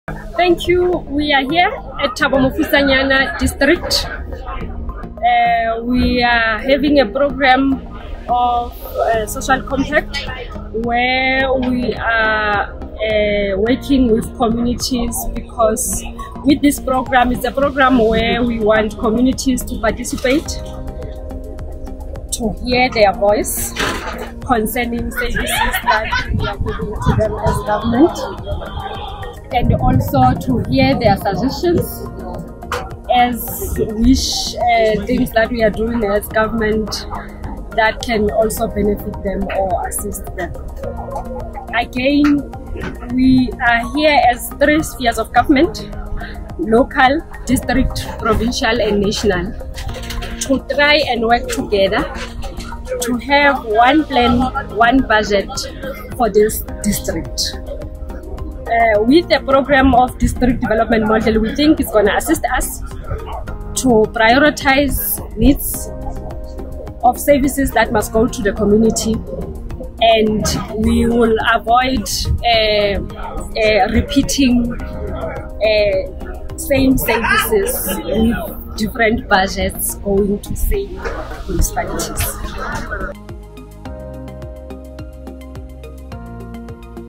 Deputy Minister in The Presidency Nomasonto Motaung joins the District Social Compact Imbizo
The Deputy Minister in the Presidency Nomasonto Motaung as the National Champion of District Development Model in Thabo Mofutsanyana today joins the District Social Compact Imbizo that has been organised and led by the Premier of the FS, Mr Mxolisi Dukwana.

The Deputy Minister has along side her Provincial Champions and Counterparts, MEC Saki Mokoena and Limakatso Mahasa led a series of build-up activities engaging with communities and addressing service delivery bottlenecks across the Thabo Mofutsanyana District especially in Maluti A Phofung Local Municipality, in QwaQwa throughout the week towards the Imbizo